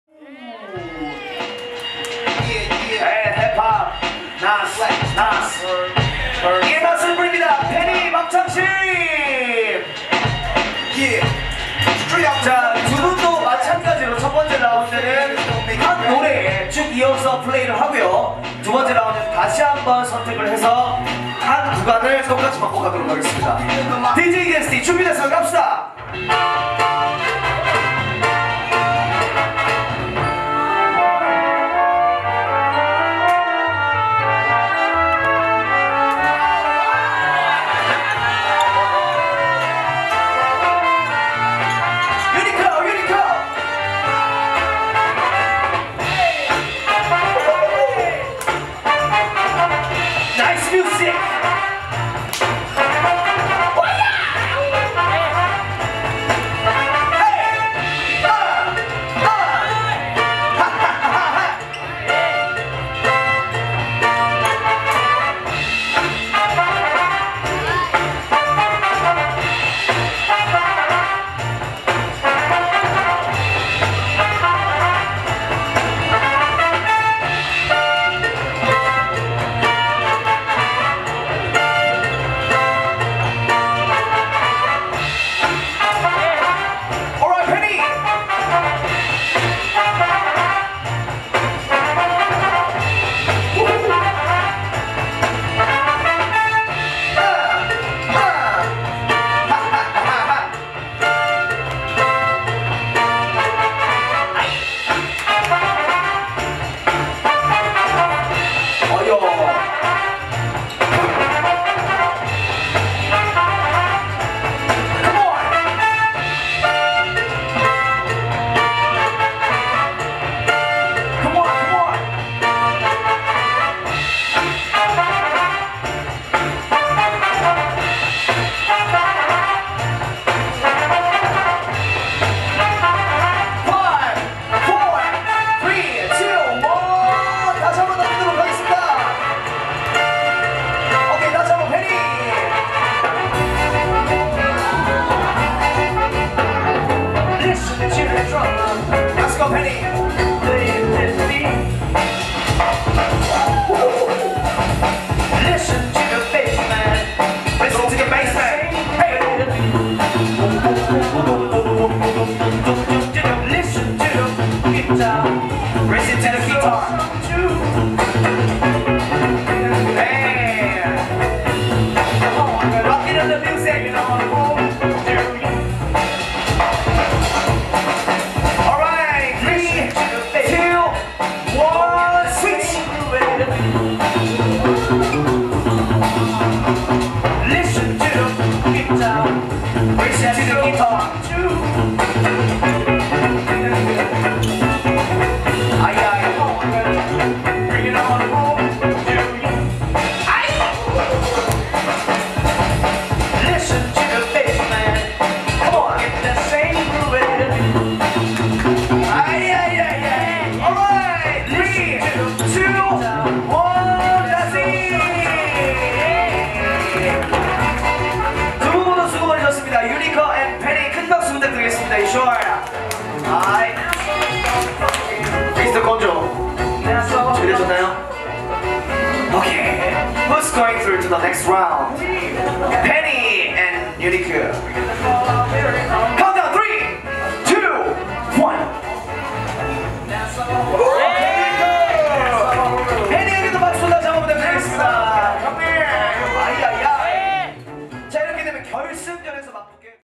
TopRock Side Top4 Uniker VS Penny l 2019 BOOGIE TRAIN VOL. 1.5 l SOUL.GRAPHIC
2019 BOOGIE TRAIN VOL. 1.5
 [Boogie Train Party]
 LOCKING | WAACKING | TOPROCK
 1 VS 1 BATTLE & SOUL TRAIN LINE

DATE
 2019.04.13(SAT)
 PLACE
 ITAEWON MOONNIGHT
 JUDGE
 LOCKING : U-BONG (Funky Street Princez)
 WAACKING : PONY (Mystique Boutique / Deep Brown)
 TOPROCK (B-BOY) : GONZO (Rivers Crew / Pxhx)
 MC
 DU LOCK (Funky Street Princez / L.N.L Crew)
 SOUL TRAIN LINE MC
 FUNNY ON (Funny On & Scar)
 DJ
 LOCKING / TOPROCK
 ZESTY (Gamblerz Crew / Cool Like that)
 WAACKING / SOUL TRAIN LINE
 JOOPOP (EarthQuake / Prodigal Crew / Run DJ Crew)
 BATTLE GUEST
 LOCKING
 LOCKING ONE (Rolling Hands)
 WAACKING
 MARID (Bitgoeul Dancers / Another Star)
 GUEST SHOW
 JACKPOT & GROOVE M
 FILM BY
 SOUL.GRAPHIC
 PHOTO
 JK HIPHOP